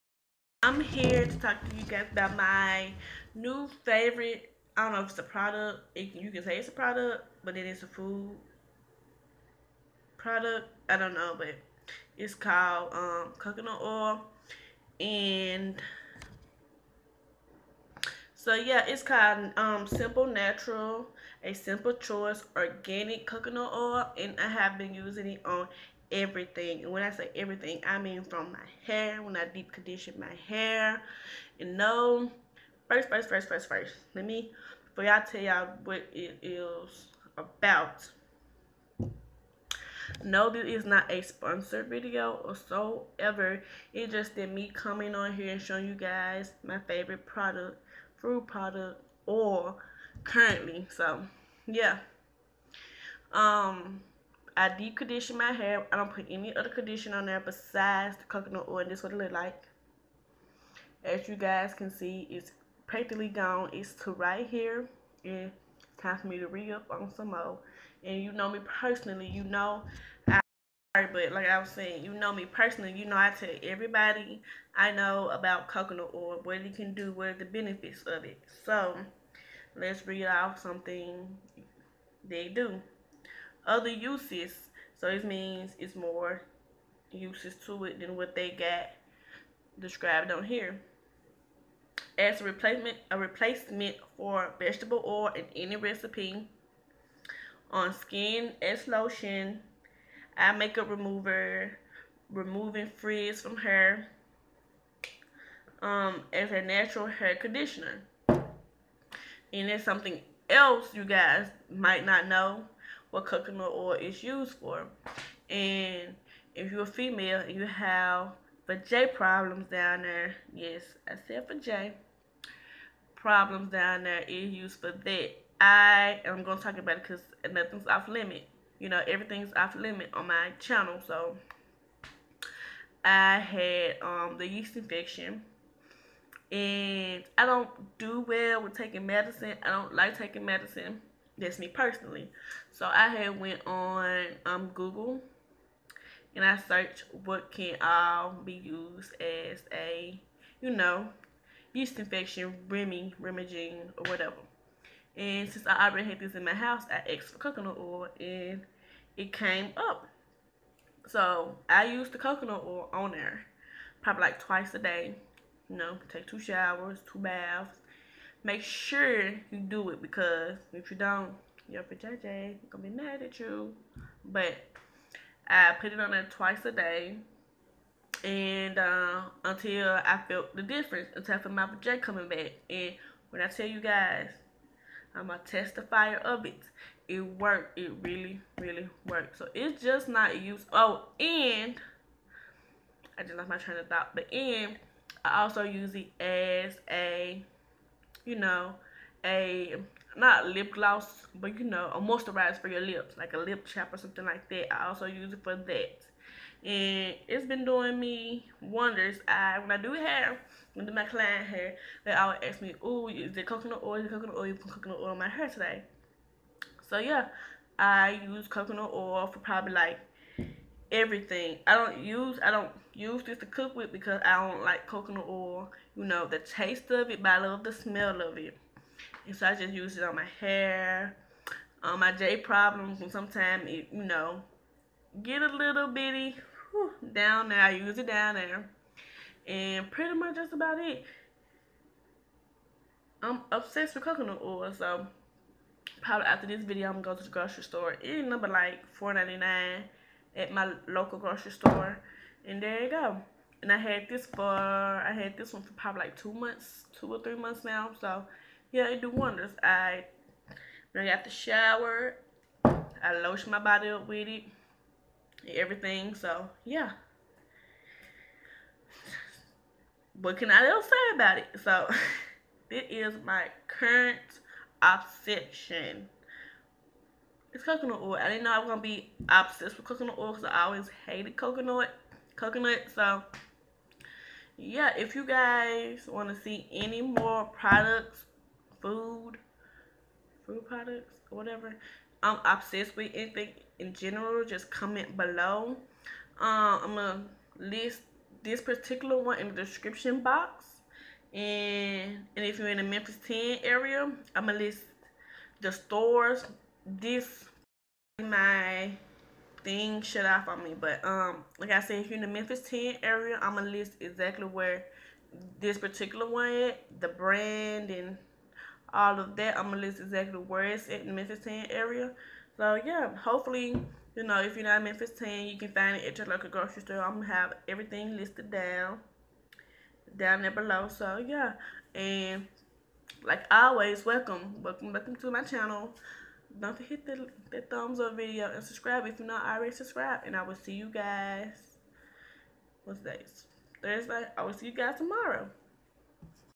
Favorite Oil/ Coconut Oil
This Is My Favorite oil i use it to condition my hair with SimplyNature Organic Coconut Oil you can get from your local food store in memphis tn at Aldi

It helps get rid of yeast infections

On skin as Lotion

Eye makeup remover

Removing frizz from hair

As a lip chap